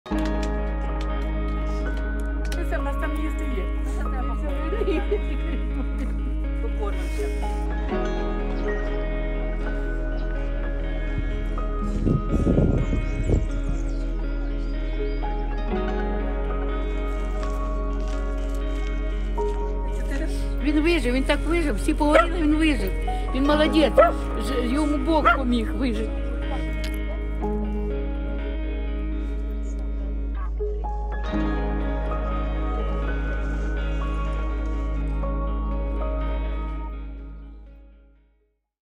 Walking the dog amid mines and missiles
‘He’s a good boy’ - What is it like to walk the dog through an abandoned Ukrainian battlefield?